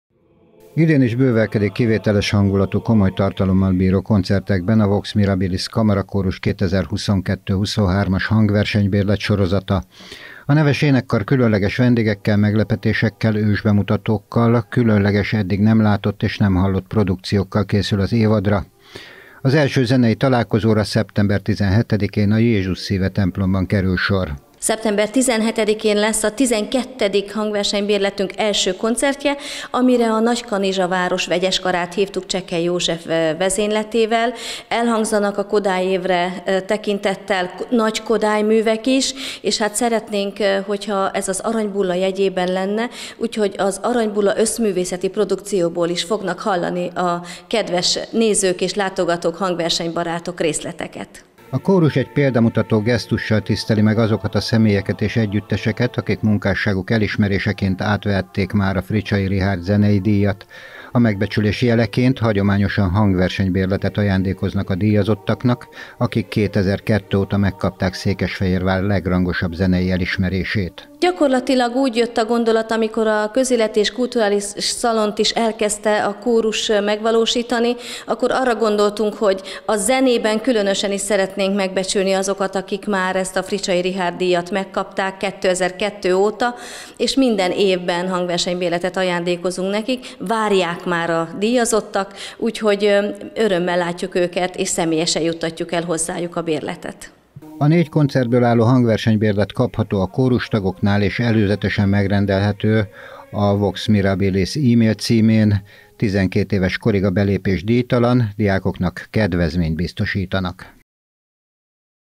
Már előrendelhető a Vox Mirabilis Kamarakórus Hangversenybérlete
Szeptember 17-én a Jézus Szíve Templomba várja a kórusmuzsika barátait a Vox Mirabilis kamarakórus. A Kórus hangversenybérletének első előadásán nagykanizsai vendégeik műsorával egészül ki a koncert.